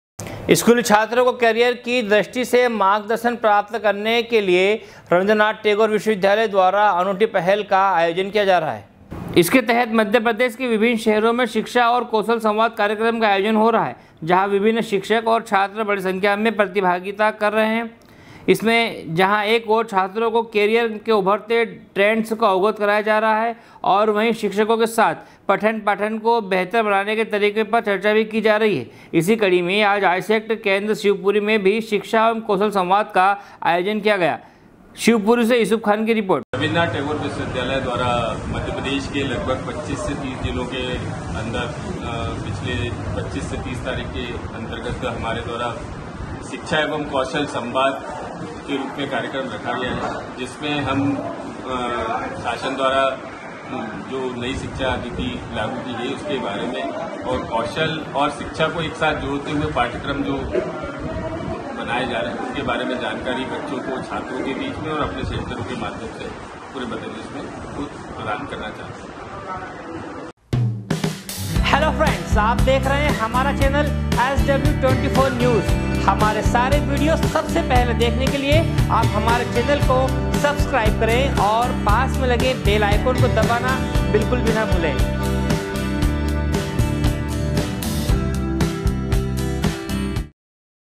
करियर की दृष्टि से मार्गदर्शन प्रदान करने के लिए रवीन्द्रनाथ टैगोर विश्वविद्यालय द्वारा अनूठी
स्कूली छात्रों को करियर की दृष्टि से मार्गदर्शन प्रदान करने के लिए रवीन्द्रनाथ टैगोर विश्वविद्यालय द्वारा अनूठी पहल का आयोजन किया जा रहा है। शिवपुरी से यूसुफ खान की रिपोर्ट